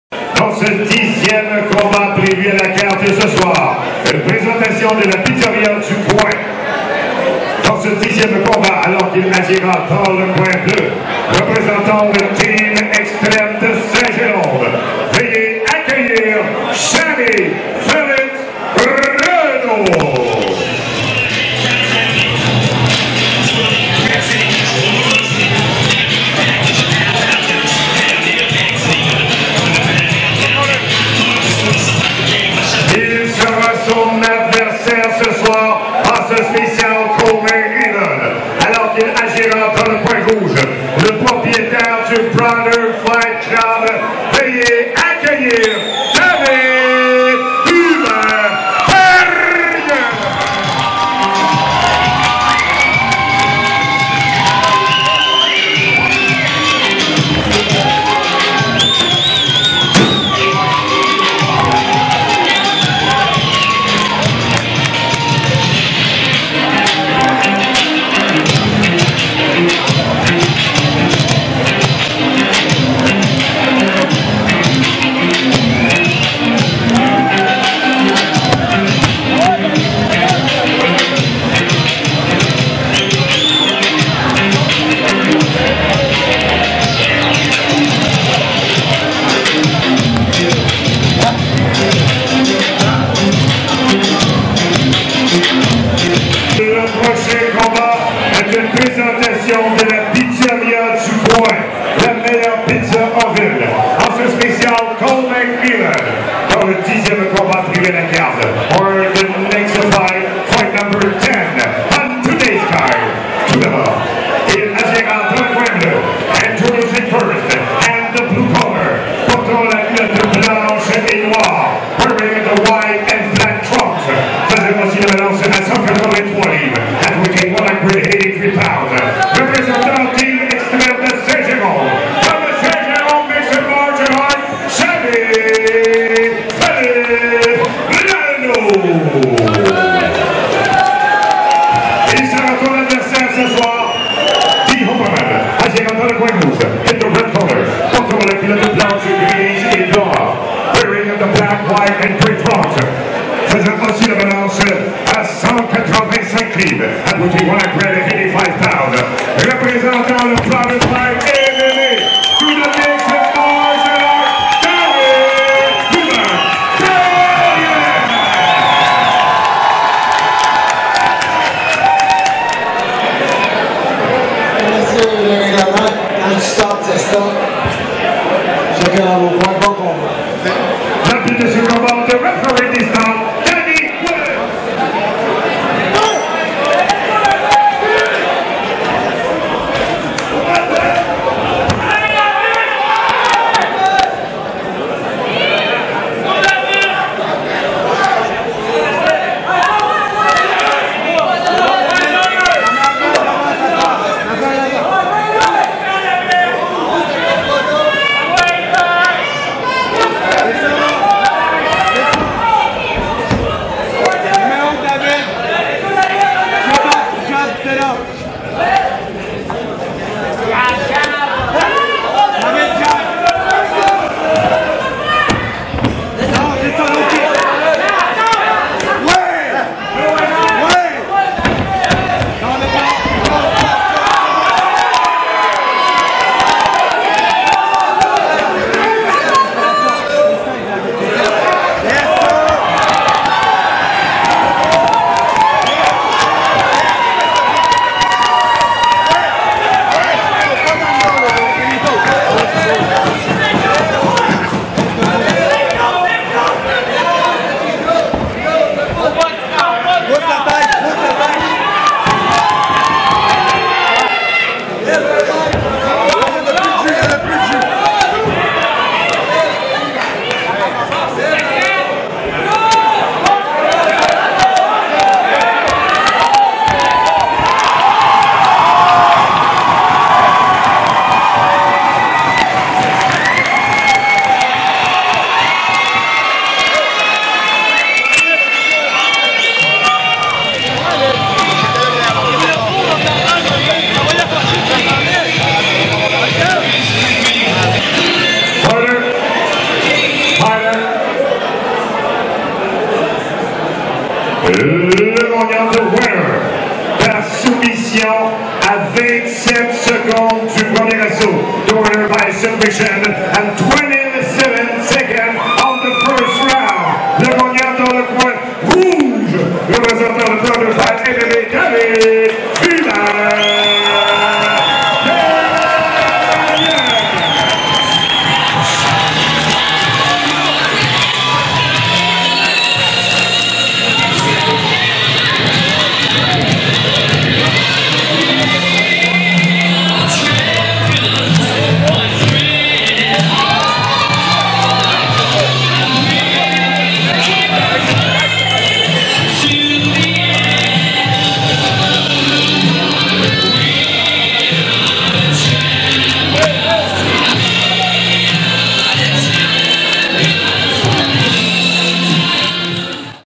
Samy phillippe Renaud VS David Hubert Therrien
Samy Phillippe Renaud affronte David Hubert Therrien  lors du Gala FIGHT NIGHT 2,  qui s'est tenu à  Trois-Rivières, Québec, Canada le 3 septembre 2016.